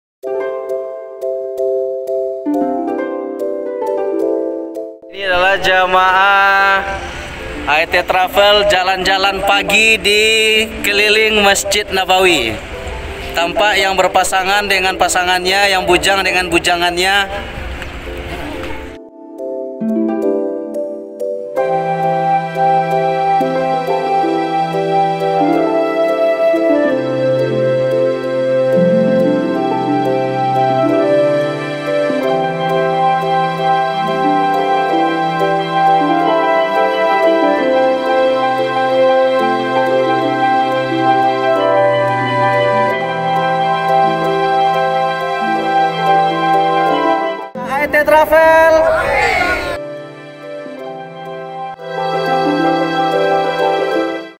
VLOG AET : Tour Masjid Nabawi
Hari ini rombongan jamaah grup III keberangkatan 3 November melaksanakan agenda tour Masjdi Nabawi.

Alhamdulillah jamaah mengikuti dengan bersemangat.